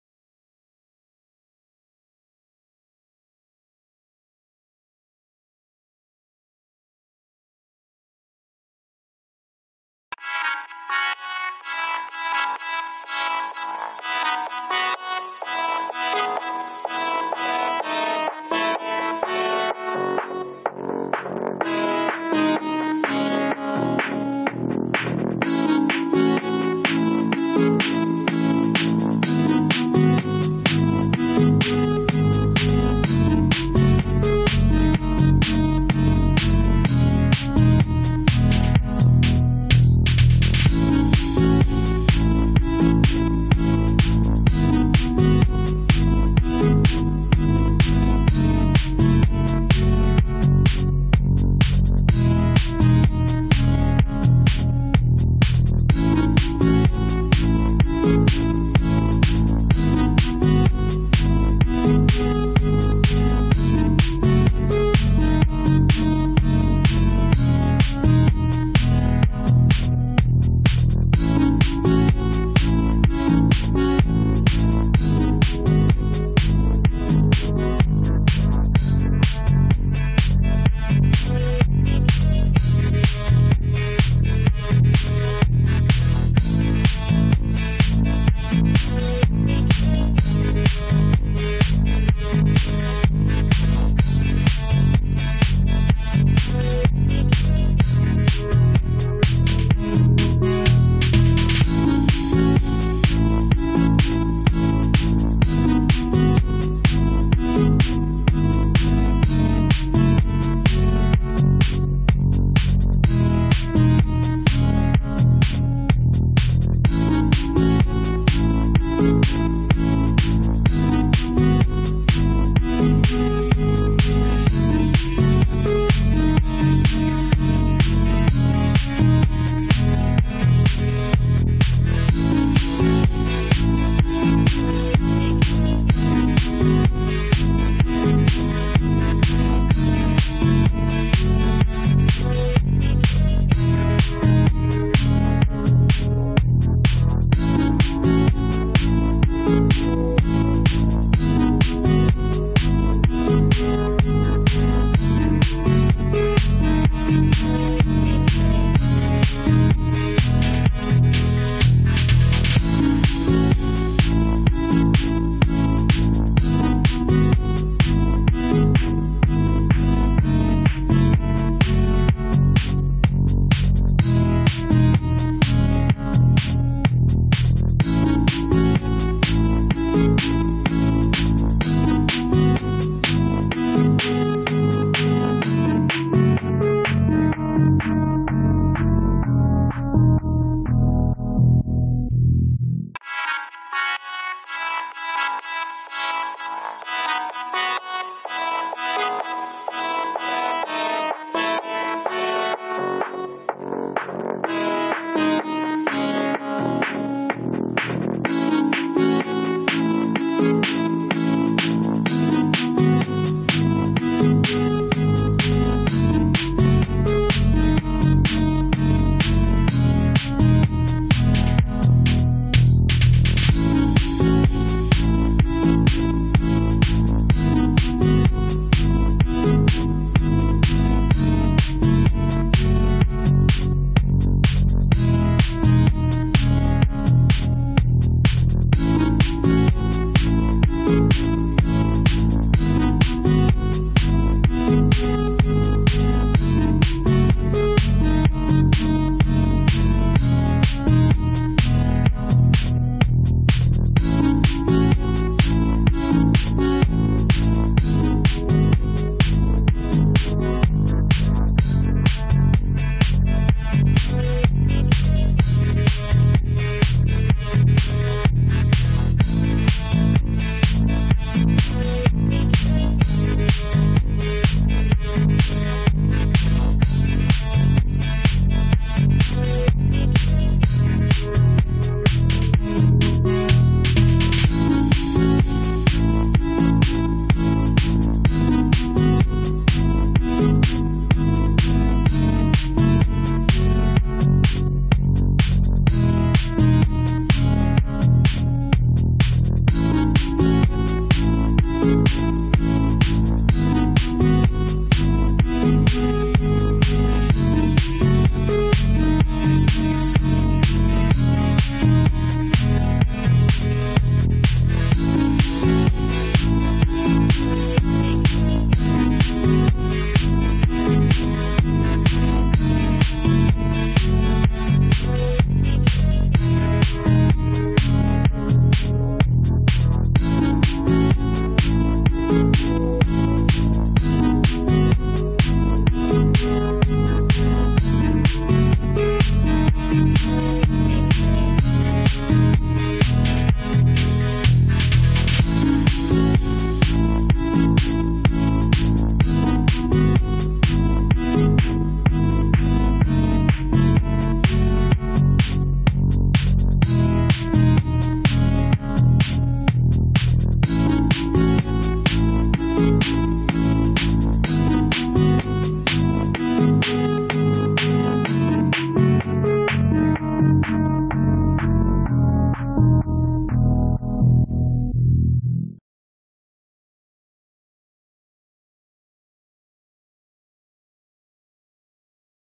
How to design circuit on zero pcb
This video shows how to design a circuit on through hole pcb.
please provide feedback by commenting on.